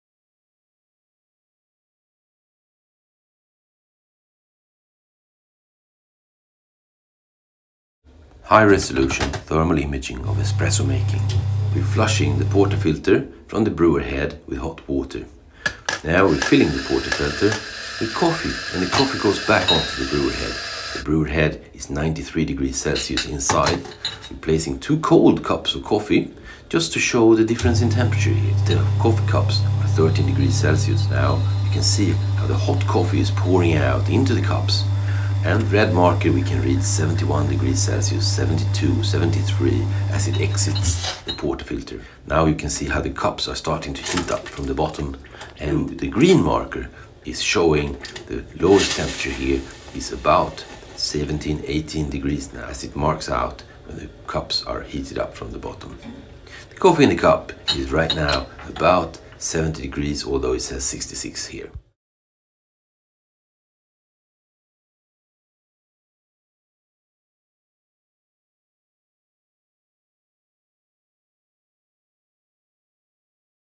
High-resolution thermal imaging of espresso making - Barista café Solo
This video shows high resolution thermal imaging of espresso making, demonstrating the involved temperatures. The imaging you see is the true thermal imaging and no fusion with photo. The machine used was an Italian semi automatic espresso machine allowed to preheat for 20 minutes.